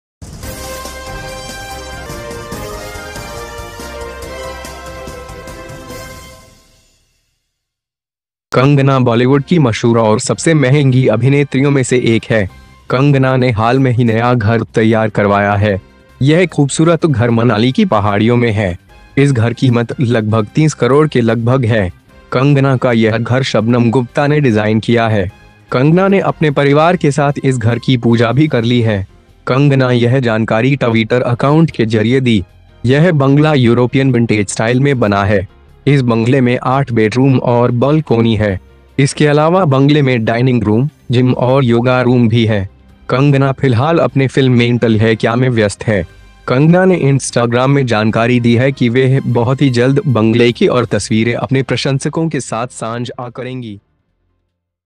कंगना का 30 करोड़ की कीमत वाला खूबसूरत घर तैयार
Kangana is one of Bollywood's most famous and most expensive actresses. Kangna has recently prepared a new house. This beautiful house is in the hills of Manali. The price of this house is approximately 30 crores.
This house of Kangana is designed by Shabnam Gupta. Kangana has also worshiped this house with her family. Kangana given this information through the Twitter account
This bungalow is made in European vintage style. This bungalow has 8 bedroom and balconies. Apart from this, there is a dining room, gym and yoga room in the bungalow.
Kangana is currently busy in her movie 'Mental Hail Ki'. Kangana has informed in the Instagram that she will soon be sharing the pictures with her fans.